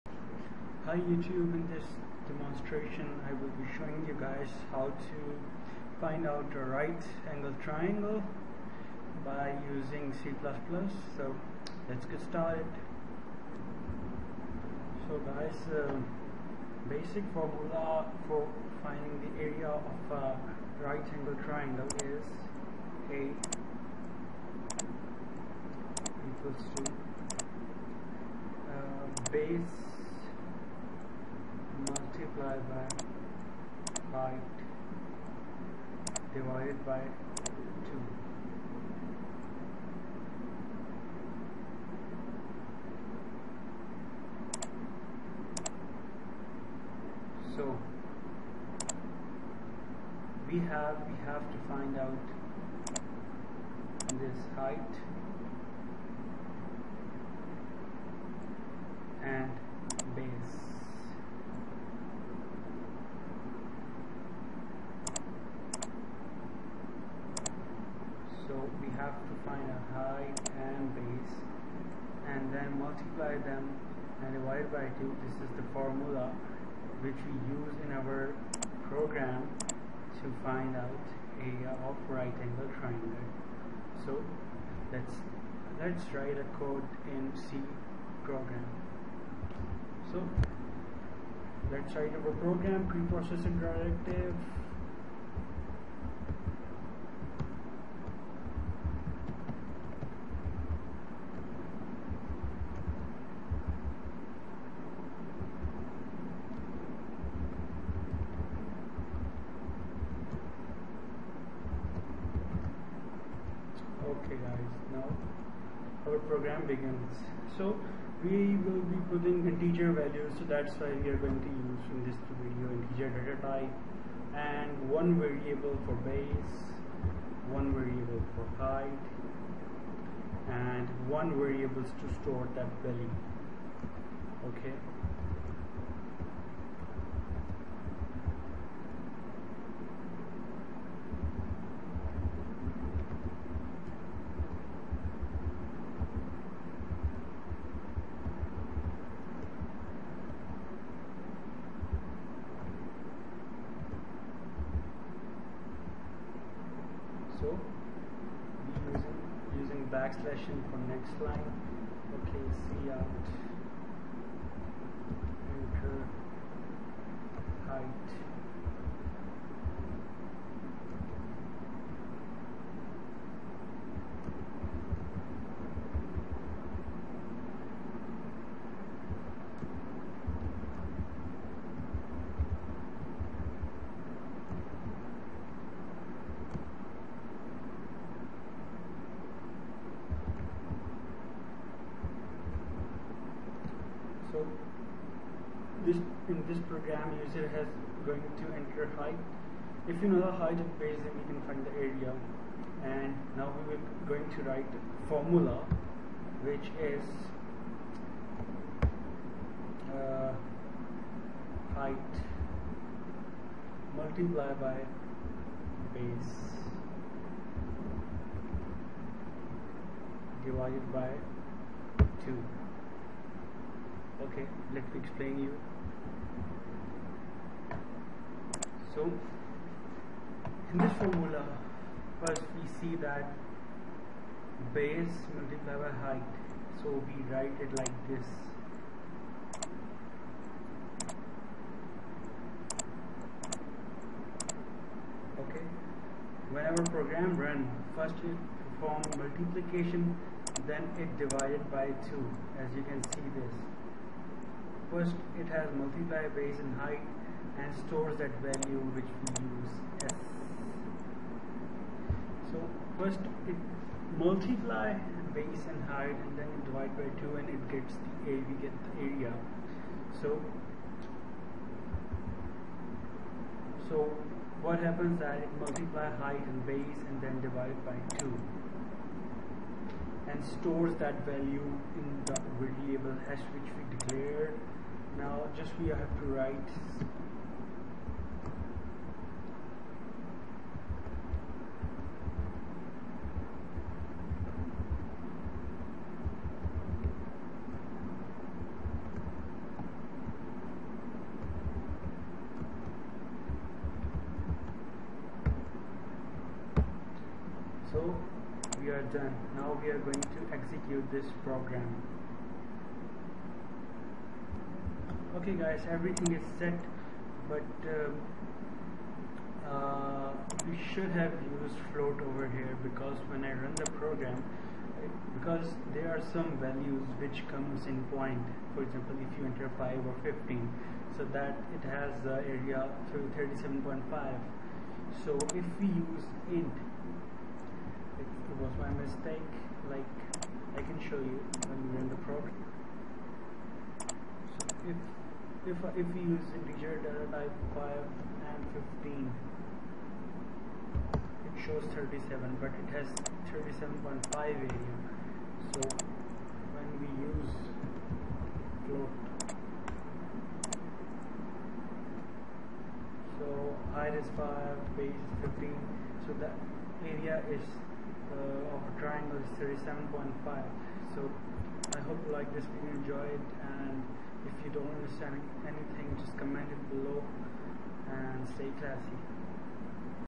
Area Of Triangle using C++
This program finds out Area of
Triangle using c++